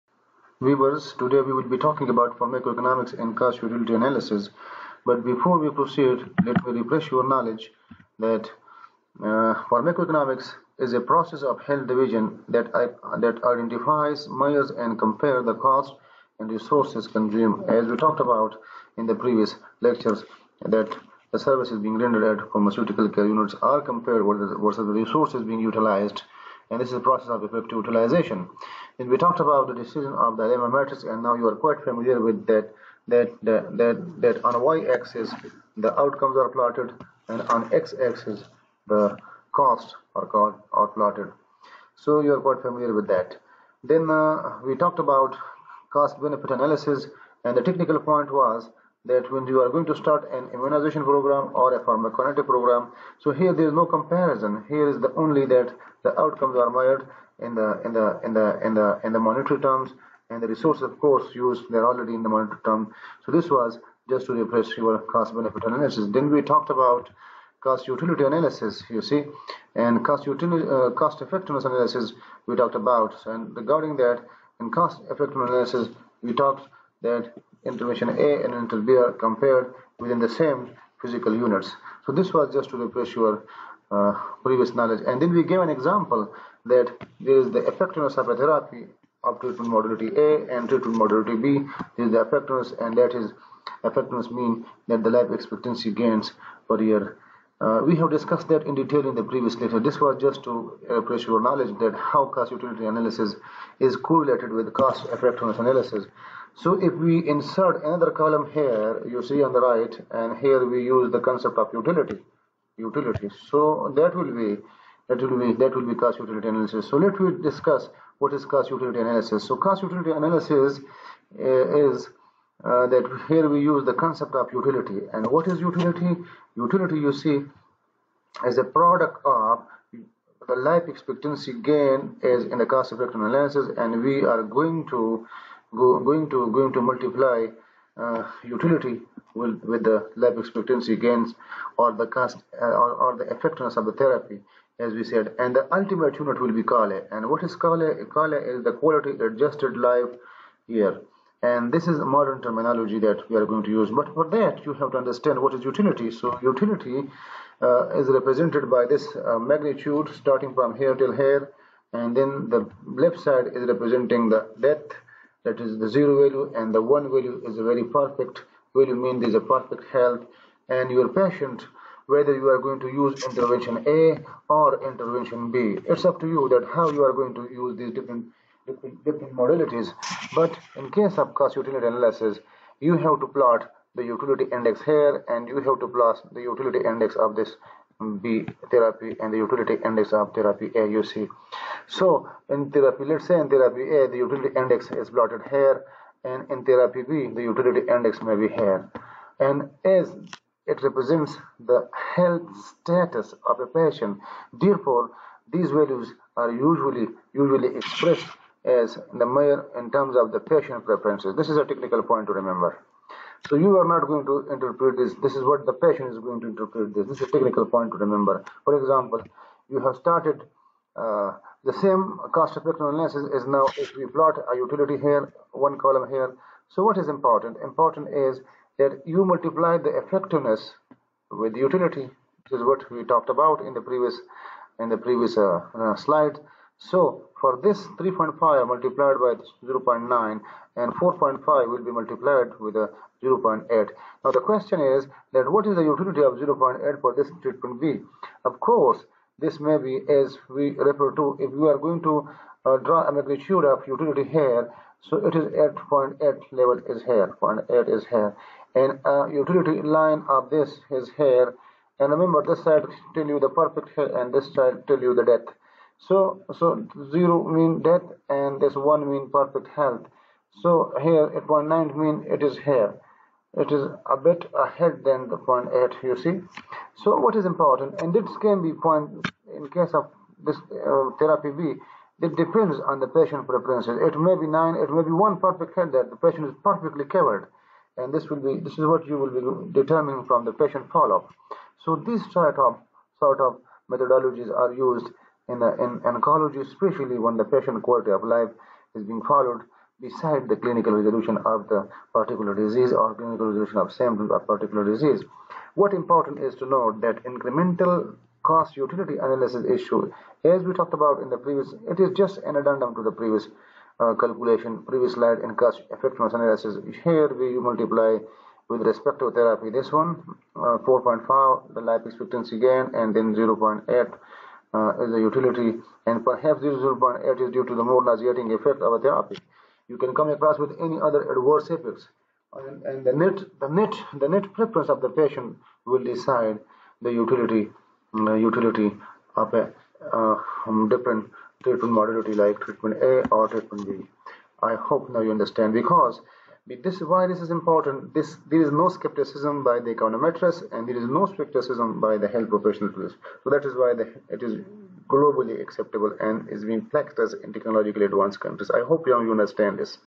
Pharmaco Economics and Cost Utility Analysis for Pharmacy, Medicine and Clinical Pharmacologists
This video describes the concepts in pharmaco-economics and cost utility analysis beside the refreshers for cost benefit analysis, cost effectiveness analysis and its implementations in oncology with unit of QALY. Target audiences are Pharm-D, Medicine and M.Phil Pharmacology programs students.